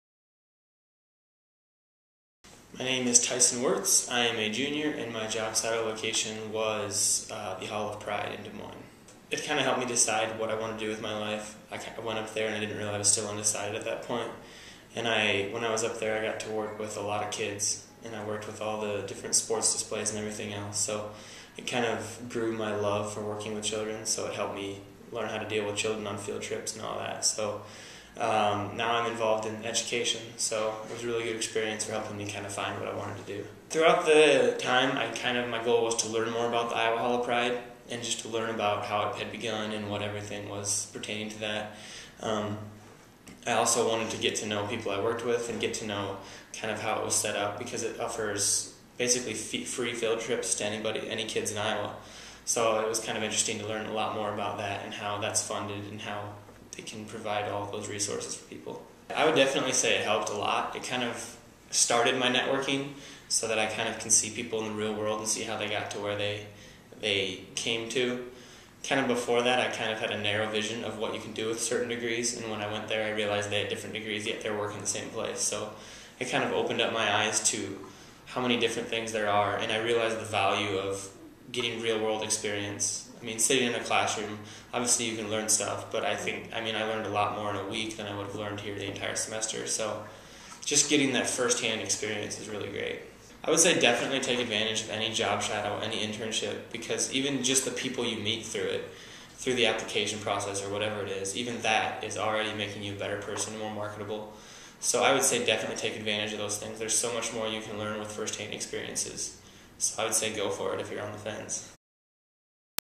Job Shadow: Tyson Wirtz
Simpson Junior Tyson Wirtz talks about his experiences in the May Term course, Career Observation, where he worked at the Iowa Hall of Pride in Des Moines.